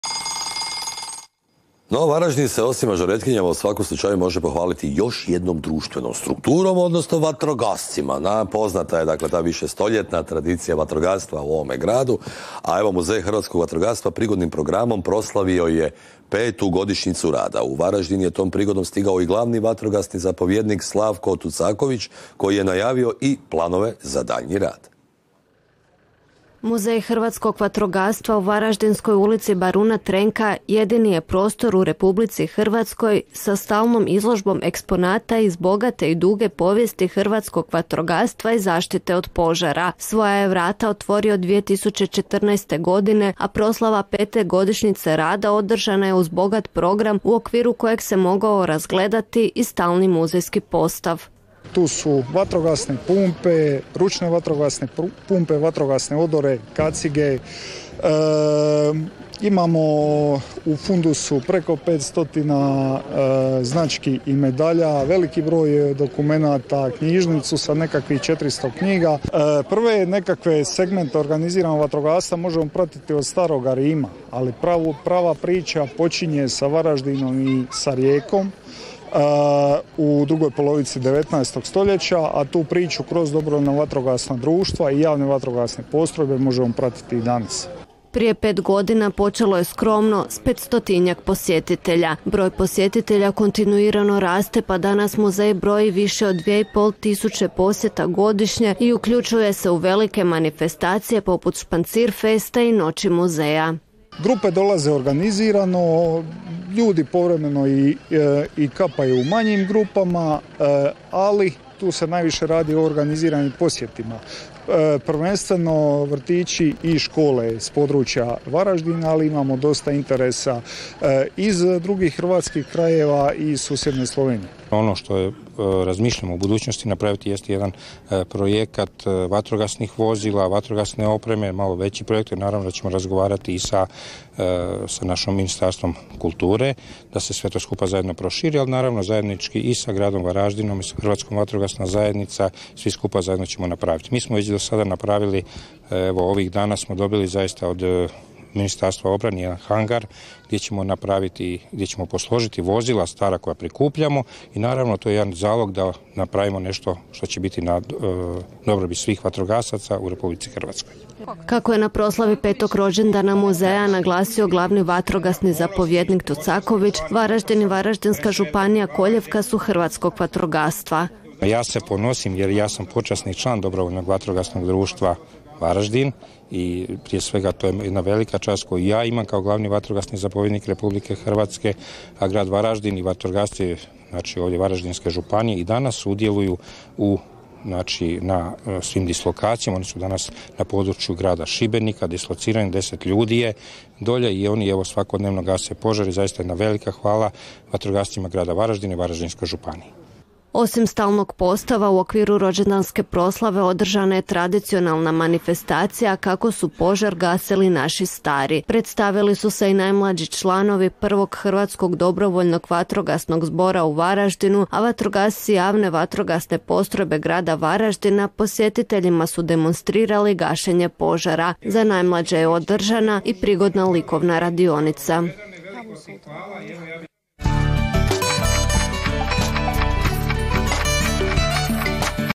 Proslavljen 5. rođendan Muzeja hrvatskog vatrogastva
Prilog o obilježavanju 5. rođendana Muzeja hrvatskog vatrogastva.
Varaždinska TV, Vekerica, 3. rujna 2019.